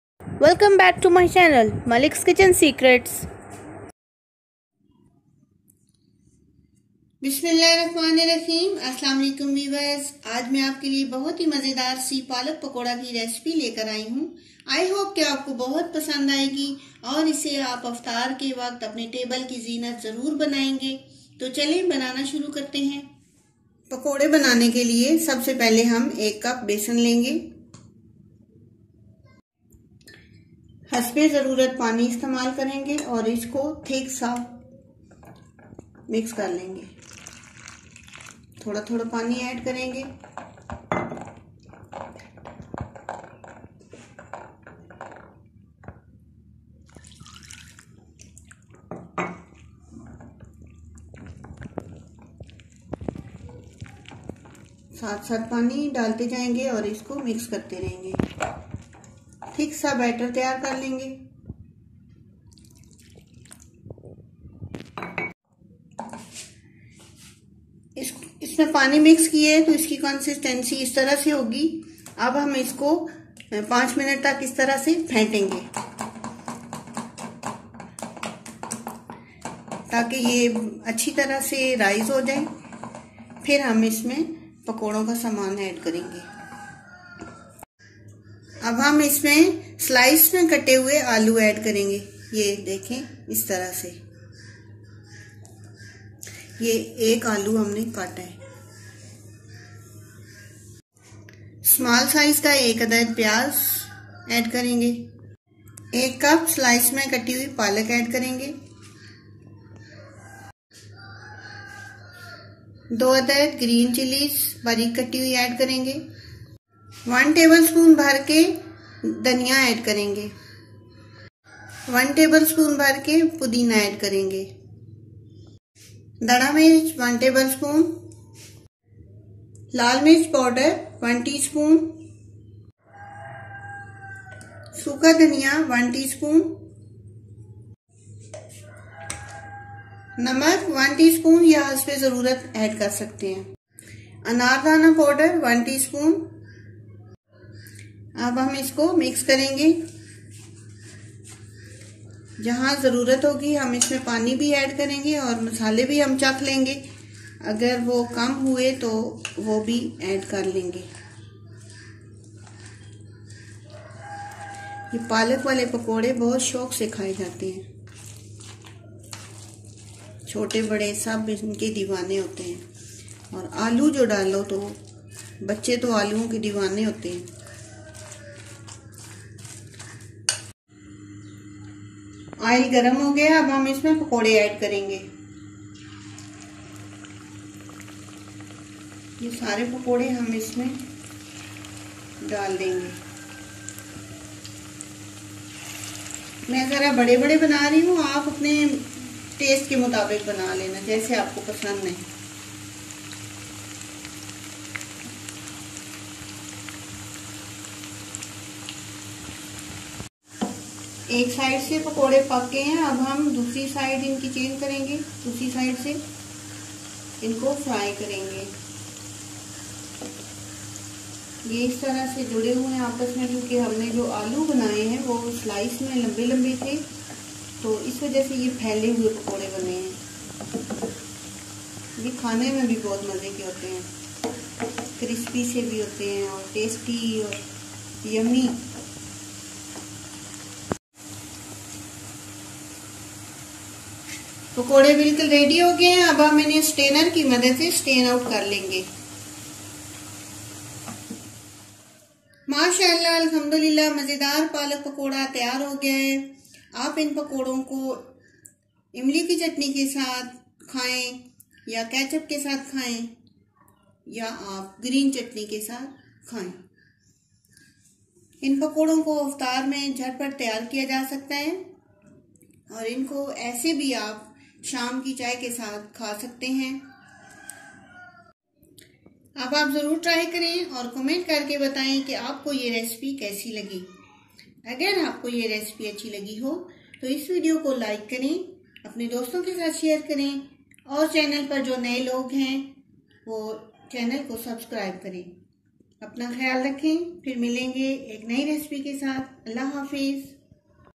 Palak Pakora Recipe | Spinach Pakora Recipe | Ramadan special
Hello friends .I hope u will be fine by the blessing  of God.  I am back with a new recipe  of palak   Pakora  .